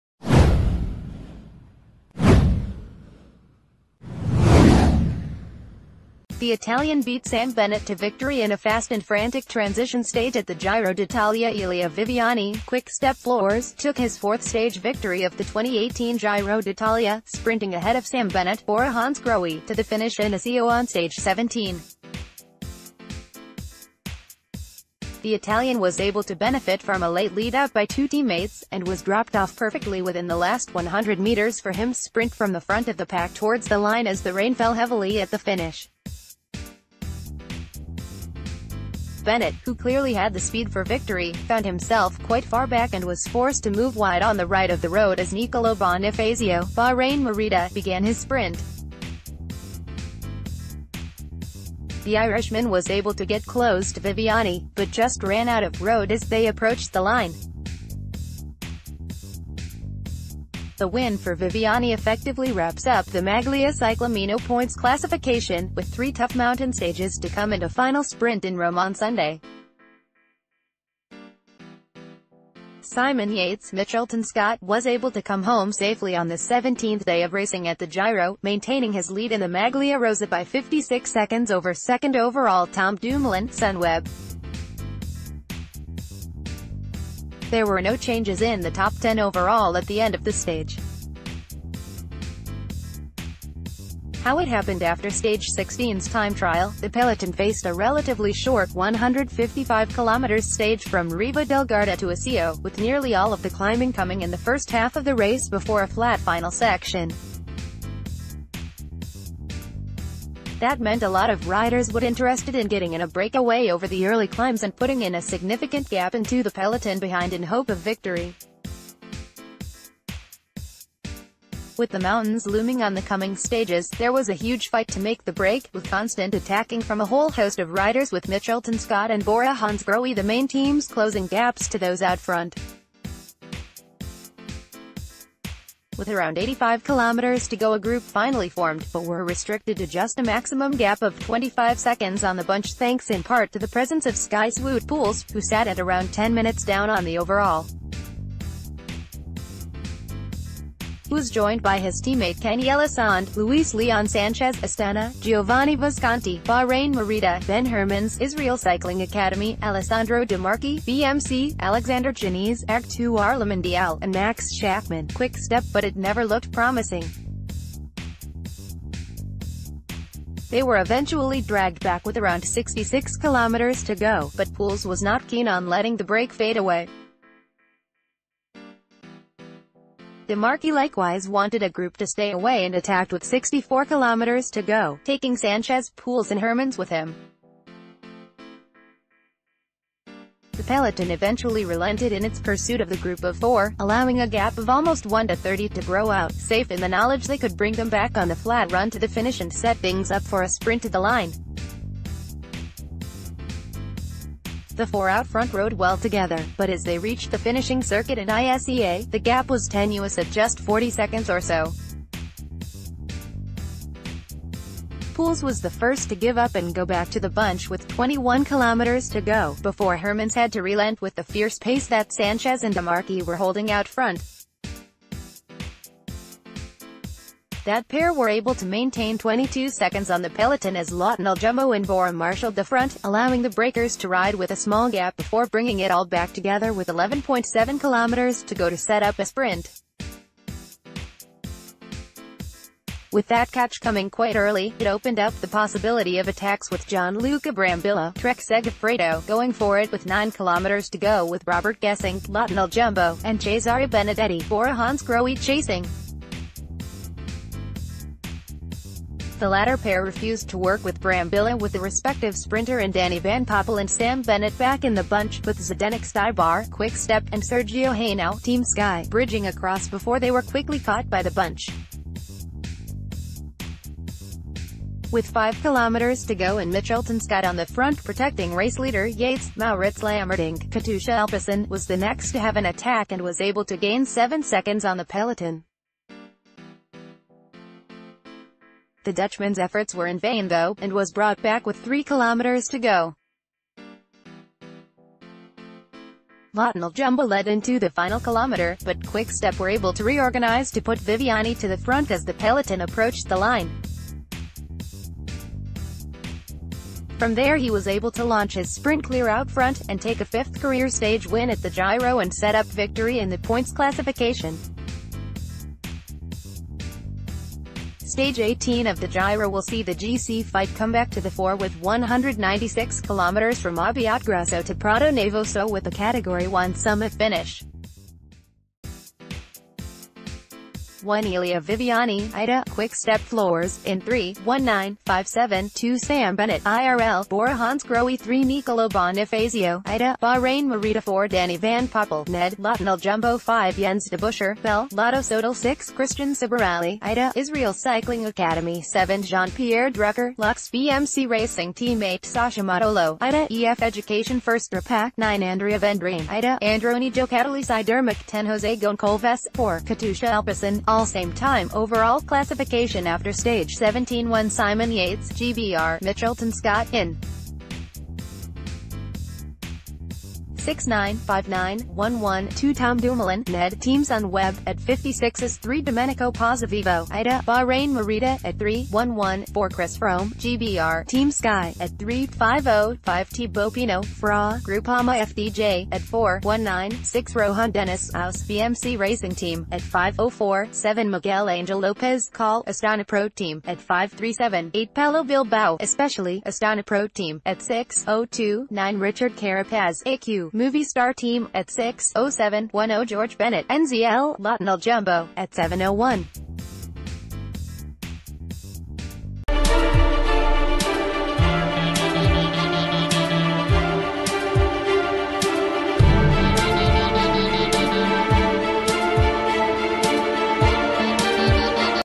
Elia Viviani sprints to fourth Giro d'Italia 2018 win on chaotic stage 17
The Italian beat Sam Bennett to victory in a fast and frantic transition stage at the Giro d'Italia Elia Viviani (Quick-Step Floors) took his fourth stage victory of the 2018 Giro d’Italia, sprinting ahead of Sam Bennett (Bora-Hansgrohe) to the finish in Iseo on stage 17. The Italian was able to benefit from a late lead-out by two team-mates, and was dropped off perfectly within the last 100 metres for him sprint from the front of the pack towards the line as the rain fell heavily at the finish.